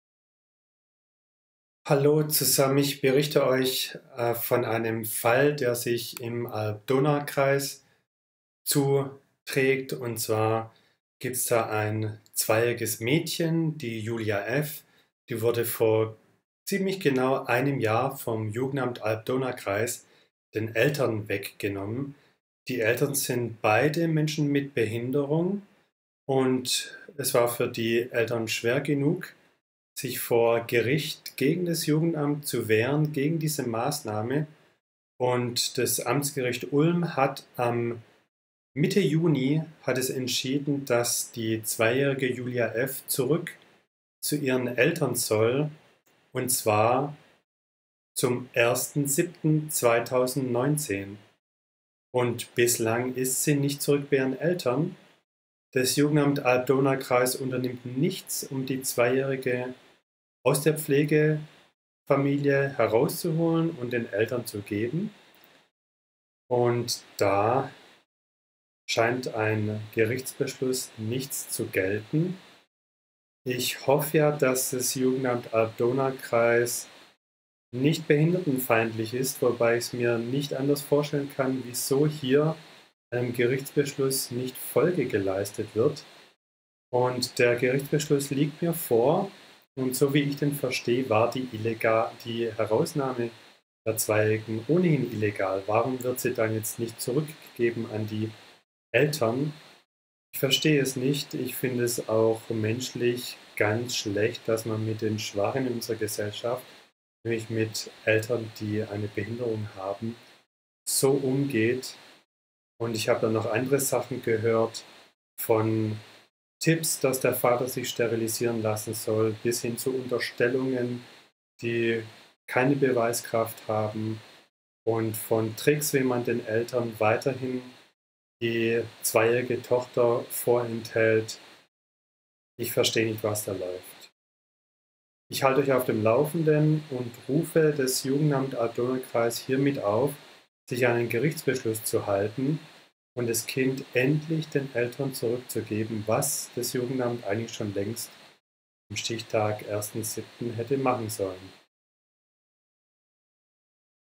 Fall Julia F.: Jugendamt Alb-Donau-Kreis gibt Eltern mit Behinderung Kind nicht zurück
Das Jugendamt Alb-Donau-Kreis missachtet seit über 3 Monaten einen Gerichtsbeschluss, dass die zweijährige Julia F. zum 1. Juli 2019 ihren Eltern mit Behinderung zurück zu geben ist.

Unterstütze uns finanziell, damit wir über noch mehr Fälle berichten können

Alle Schilderungen entsprechen der persönlichen Meinung der Personen im Video.